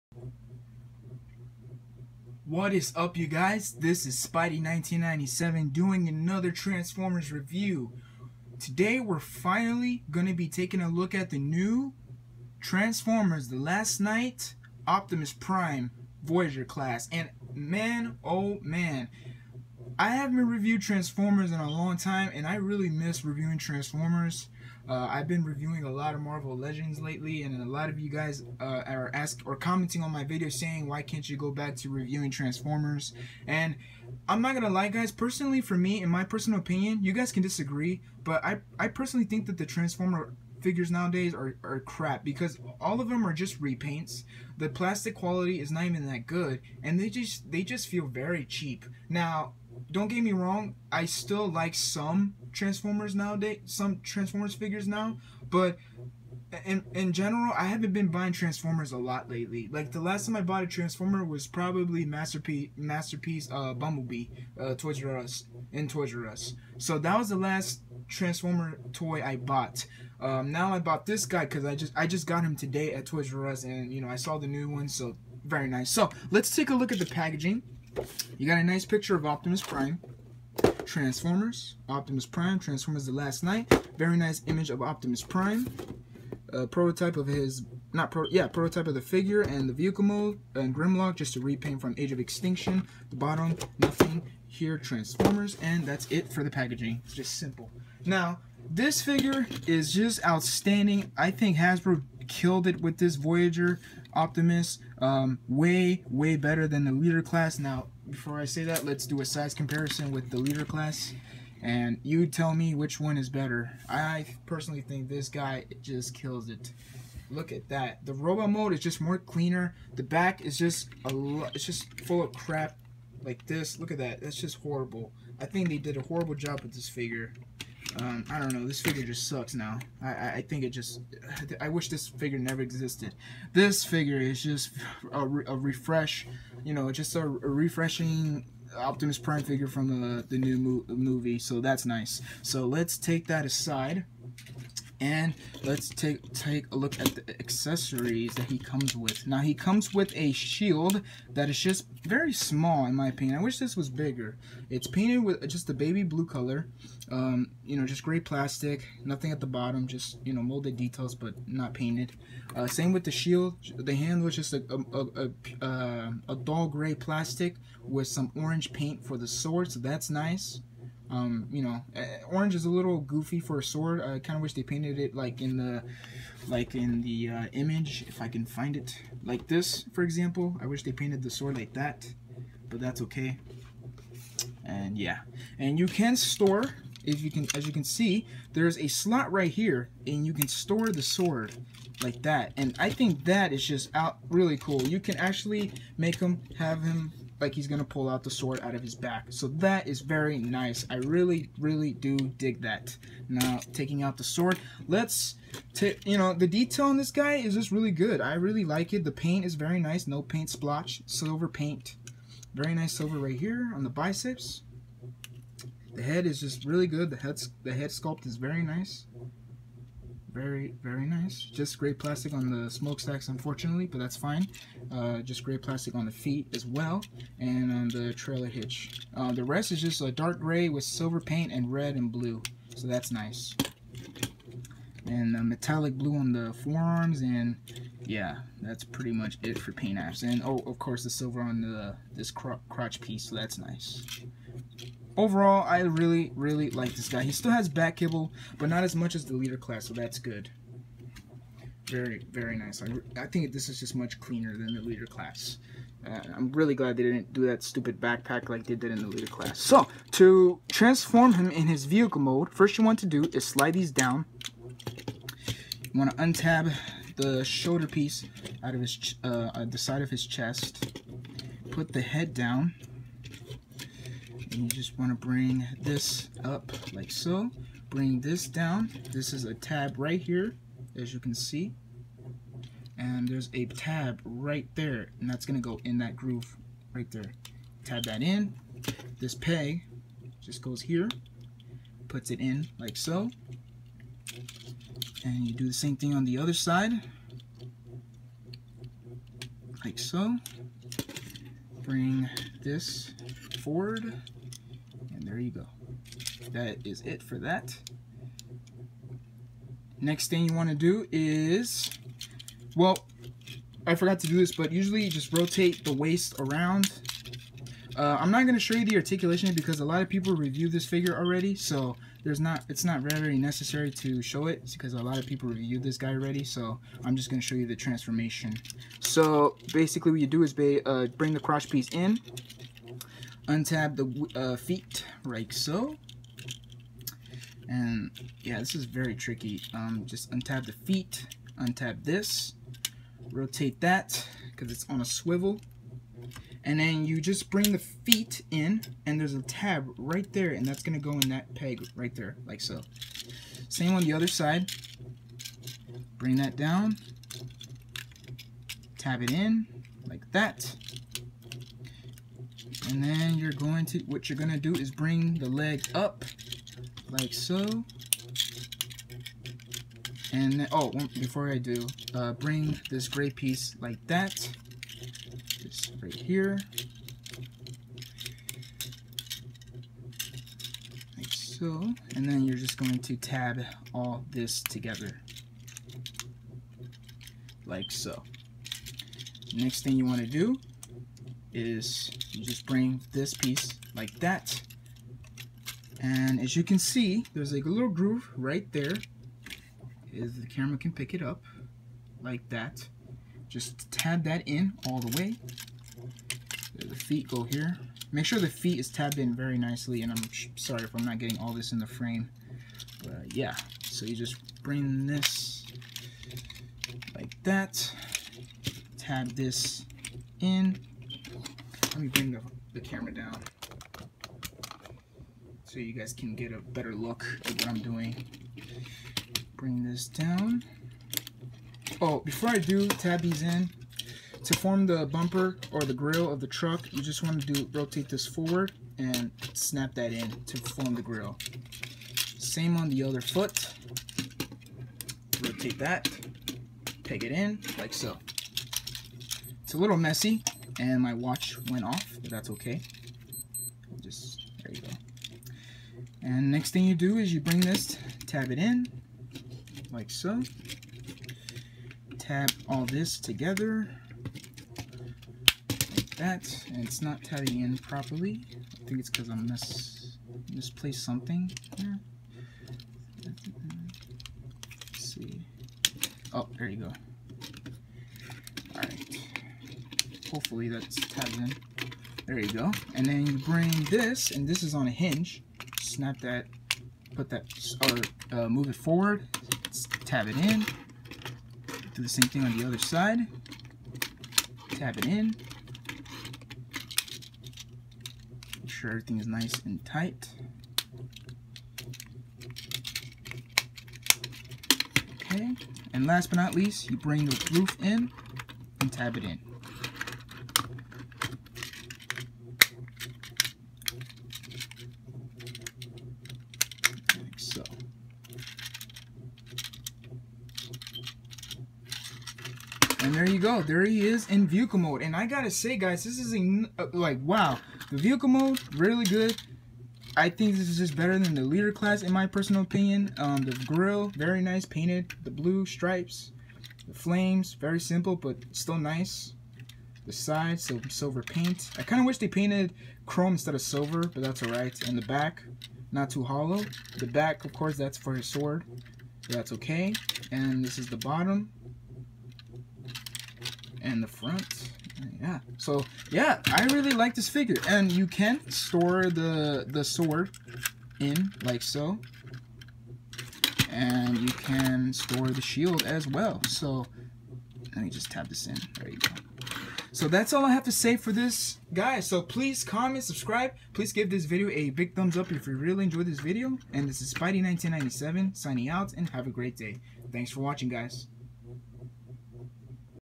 Transformers The Last Knight Voyager Class Optimus Prime! (Premier Edition) Figure Review
Whether you hate bayformers or not, this is a really great voyager Optimus Prime figure!!!